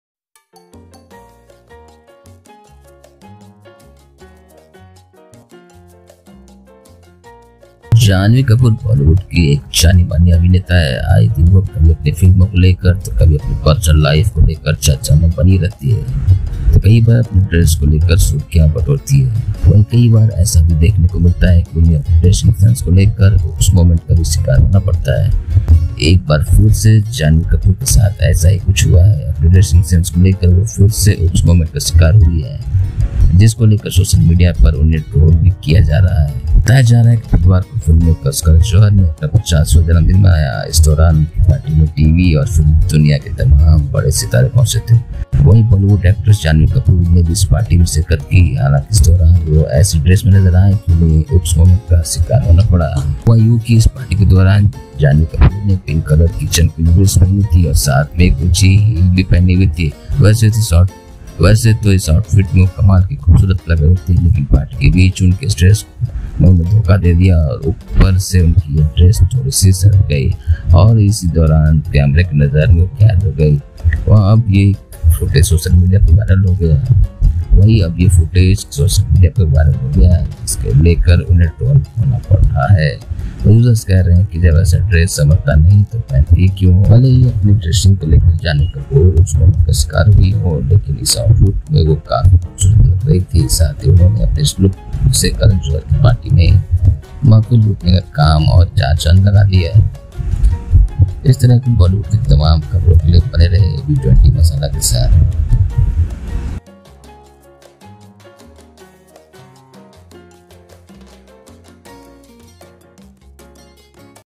Janhvi kapoor oops moment at party | B20masala
Janhvi kapoor oops moment at party:Janhvi Kapoor attended Karan's birthday bash in a shimmery bright pink dress and completed her look with on-point make-up. The actress looked quite uncomfortable in the dress and also became the victim of an oops moment.

BollywoodNews,
BollywoodUpdates,
BollywoodCelebs,
BollywoodLatest,